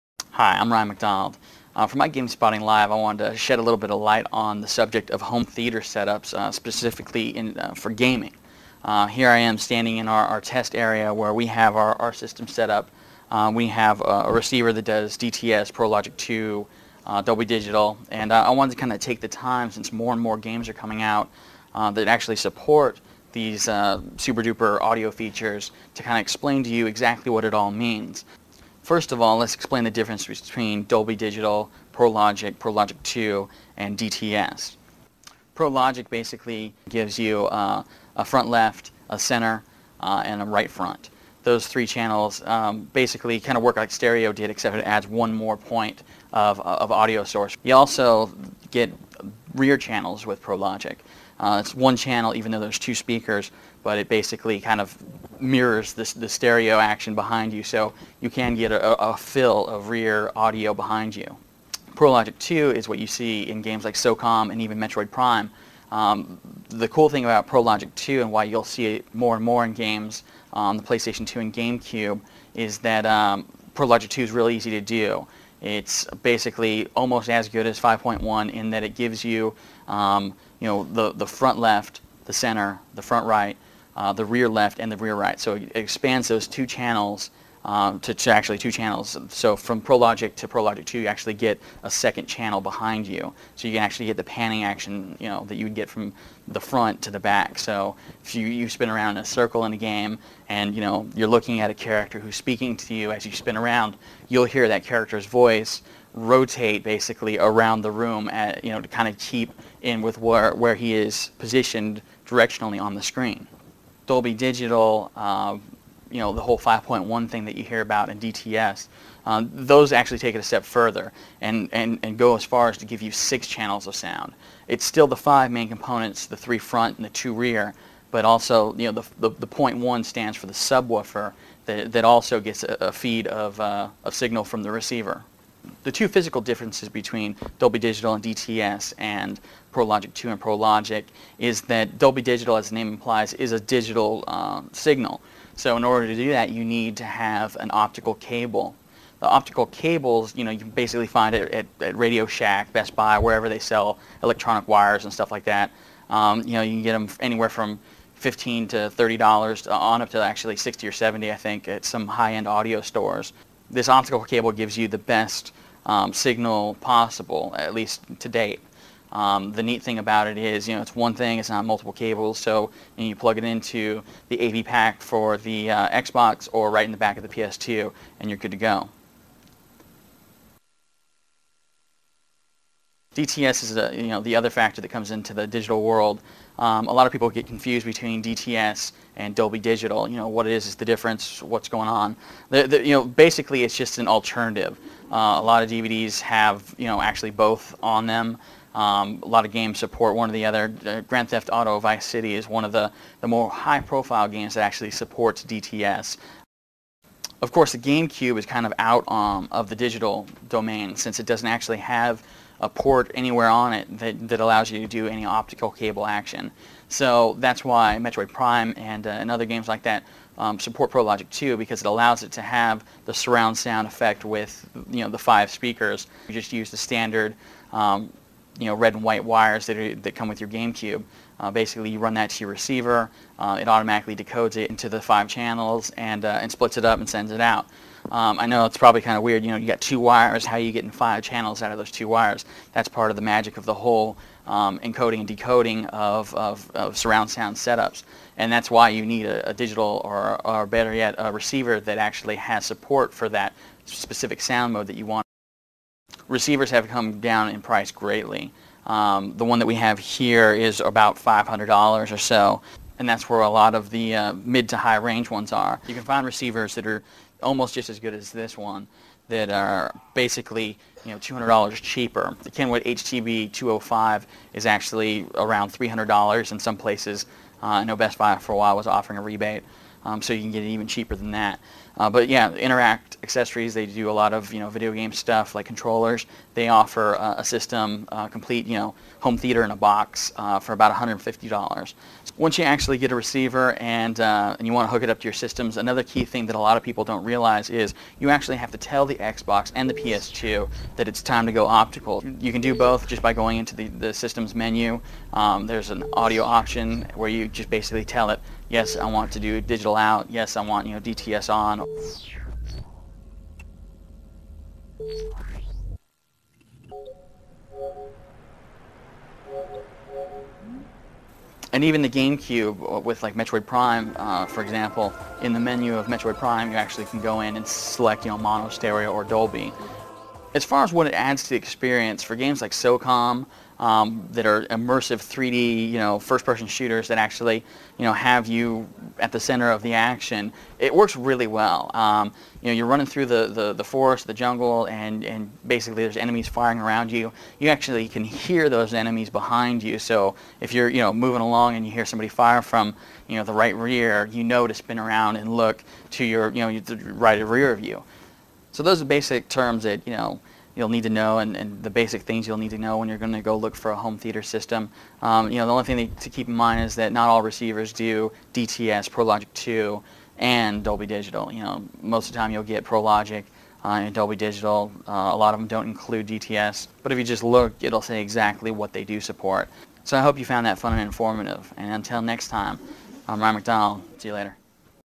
Sound Advice
Ryan sets the record straight about surround sound and walks you through the basics of how to get it set up in your home.